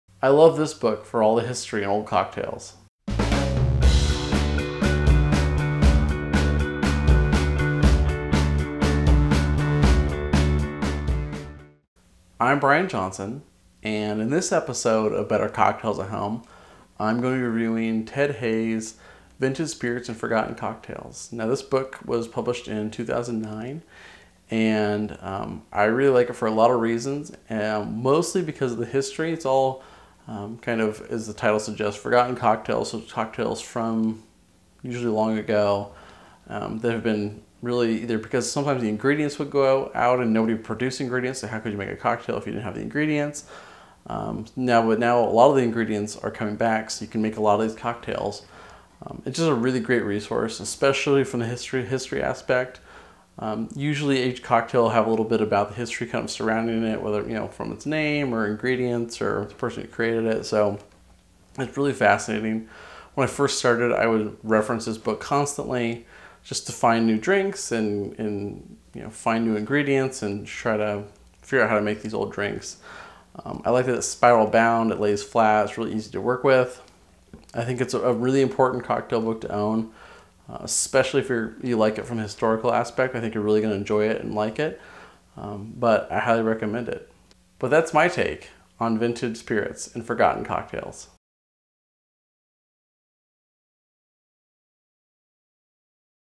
Vintage Spirits and Forgotten Cocktails book review from Better Cocktails at Home
If you don't already own Ted Haigh's, aka Dr. Cocktail, Vintage Spirits and Forgotten Cocktails, then good for you. You have one of the better cocktail books out there. This version is an update to his original book (which is long out of print) and is superior in every way. File this one under "must own!"